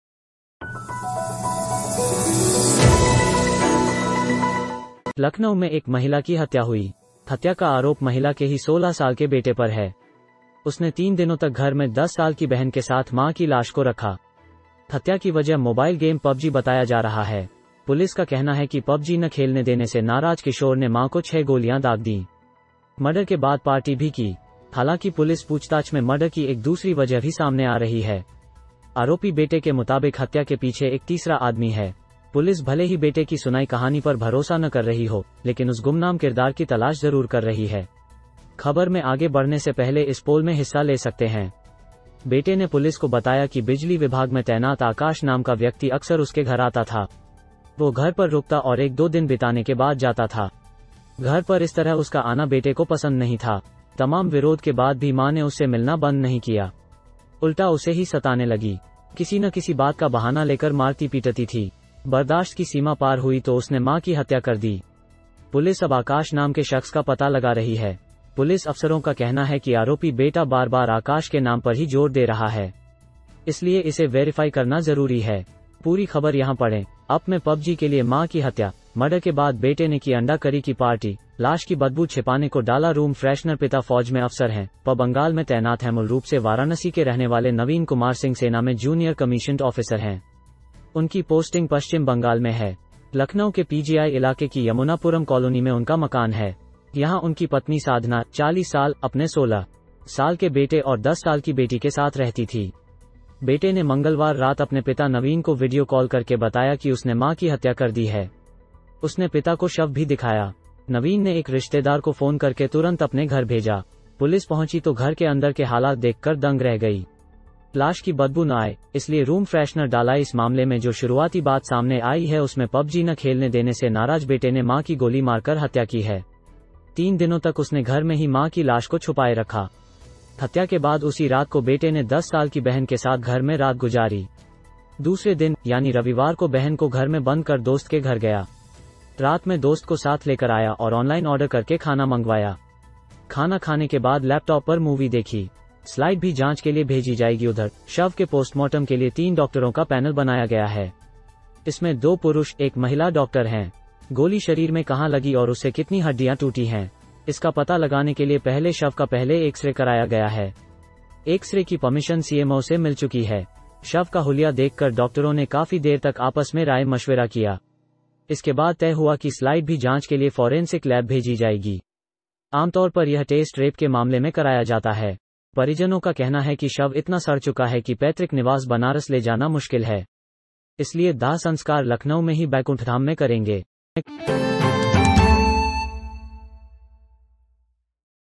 Mother murdered in Lucknow, PUBG and anonymous characters: accused son said- One person did not like
लखनऊ में मां की हत्या, PUBG और गुमनाम किरदार: आरोपी बेटा बोला- घर पर एक व्यक्ति का आना पसंद नहीं था, विरोध पर भी मां नहीं मानीलखनऊकॉपी  - Mother murdered in Lucknow, PUBG and anonymous characters: accused son said- One person did not like to come to the house, mother did not even do not like protests